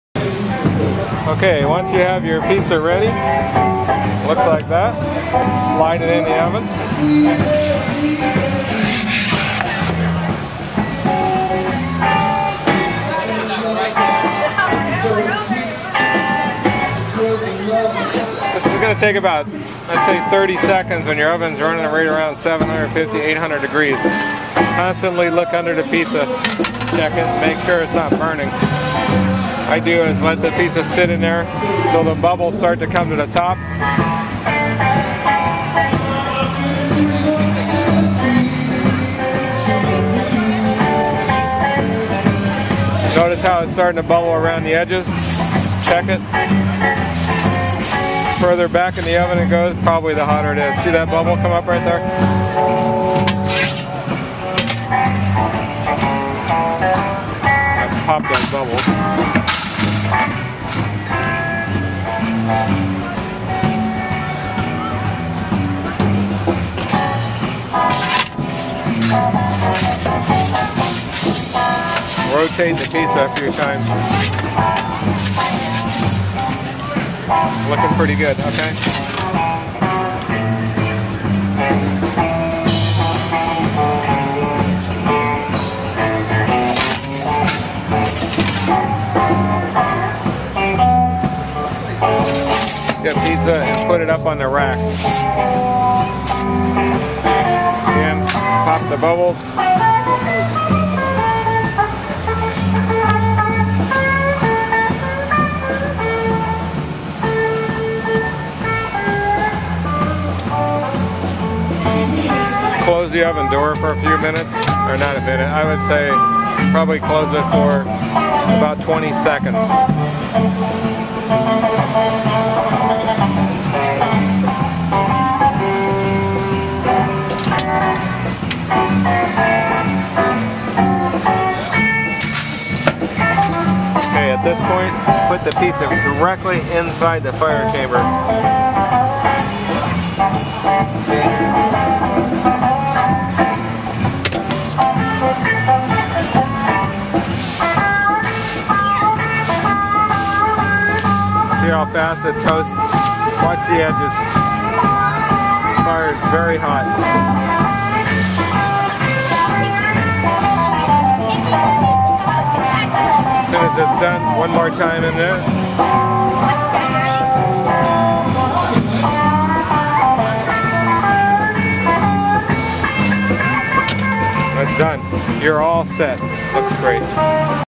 pizza in 3 minutes cardinali wood fire pizza
cardinali wood fire pizza  cook in less than 4 minutes.